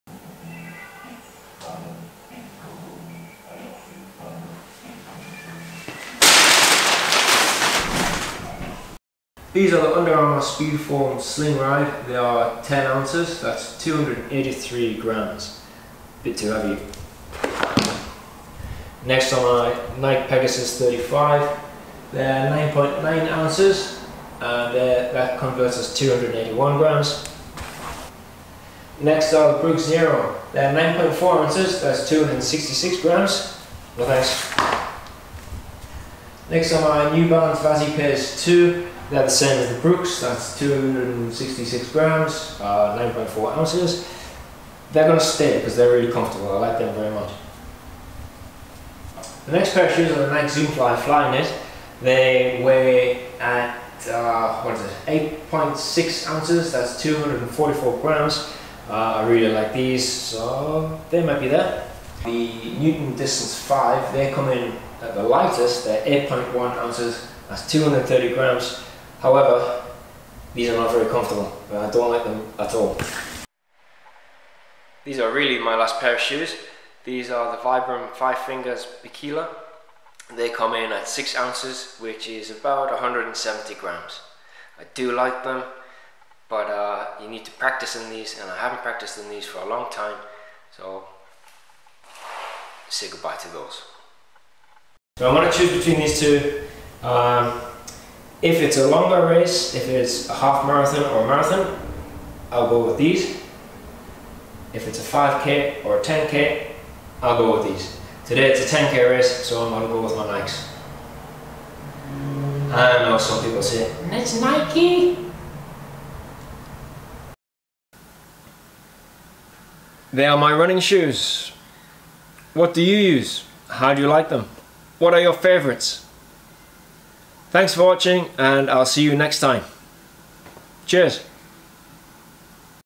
What Shoes I Use and How Much They Weigh
What running shoes do you use? What are you favourites?

Camera used : Nikon D3500.
Under Armour Speedform Slingride
Brooks Neuro
Nike Pegasus 35
New Balance Vazee Pace
Nike Zoom Fly Flyknit
Newton Distance V : Too expensive and uncomfortable. I won't recommend these.
Vibram FiveFingers Bikila